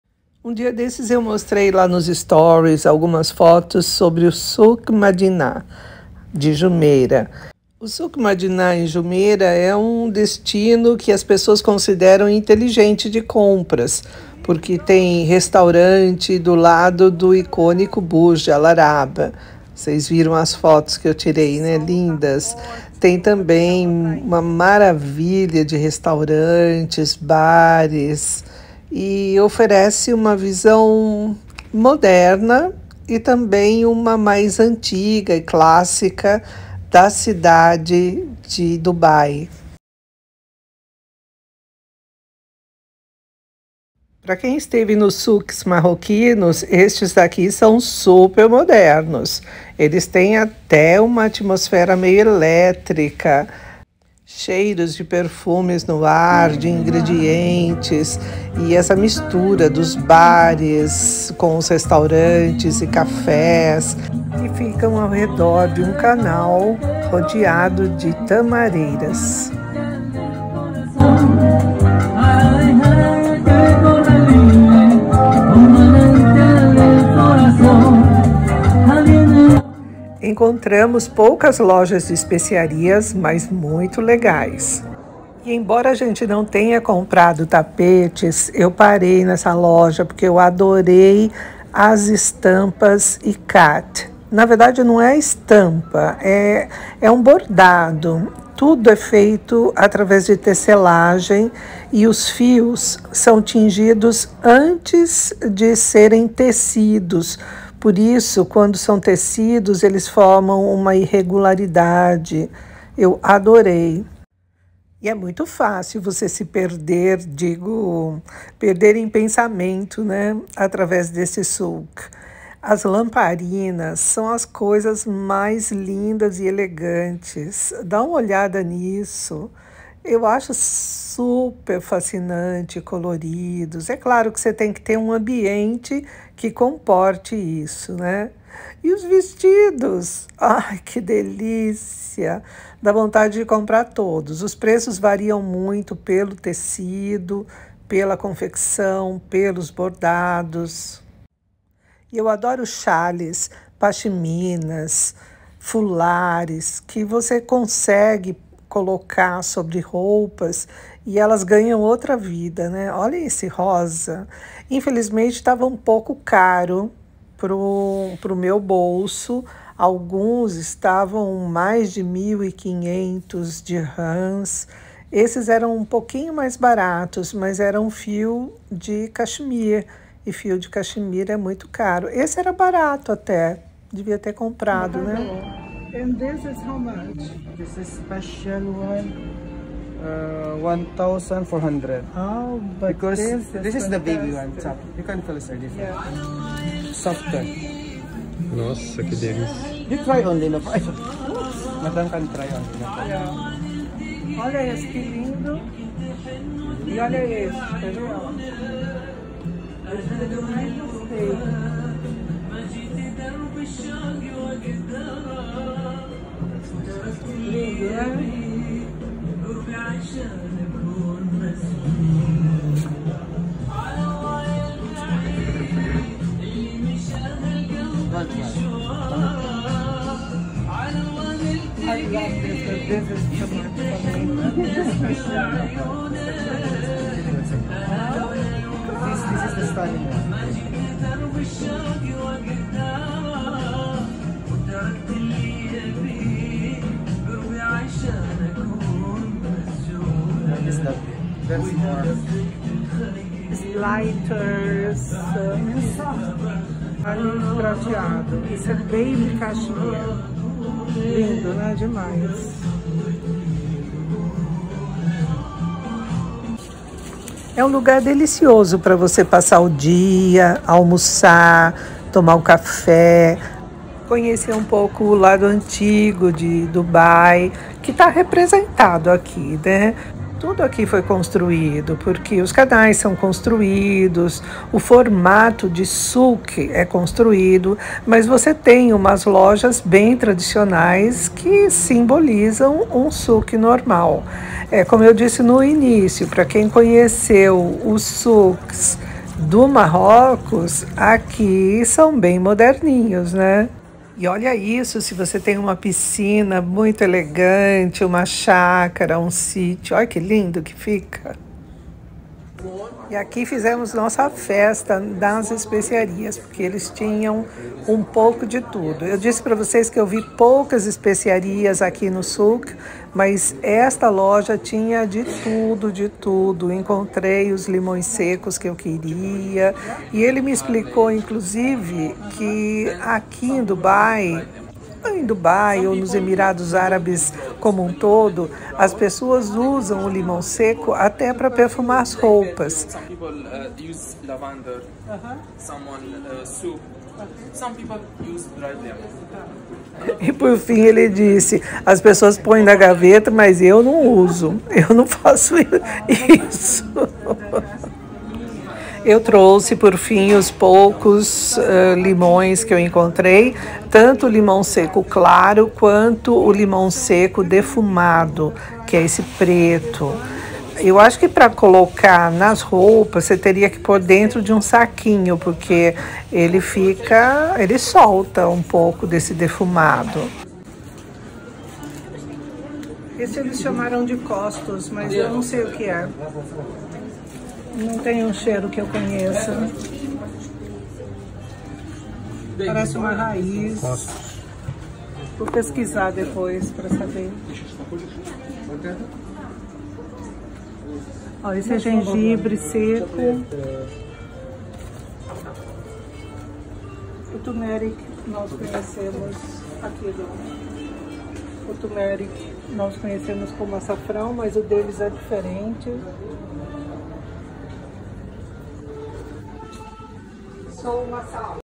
Souk Madinat Jumeira, Dubai
O Souk Madinat Jumeira, em Dubai, é um lugar que junta um pouco do antigo, mas muito do novo de Dubai.

Um lugar lindo, bastante próximo do hotel Burj Al Arab. Uma mistura incrível de cheiros, com bares, restaurantes e lojas de todos os artigos.

É um souk criado pra ser bonito, mas mantém a atmosfera dos labirintos dis souks tradicionais e as lojas que misturam artigos caros e baratos (não muito aqui).

Os restaurantes são uma verdadeira experiência gastronômica, com representantes de boa parte de países não só do mundo árabe, espalhados ao longo de um canal super agradável.

Espero que possam ter uma ideia com este compilado.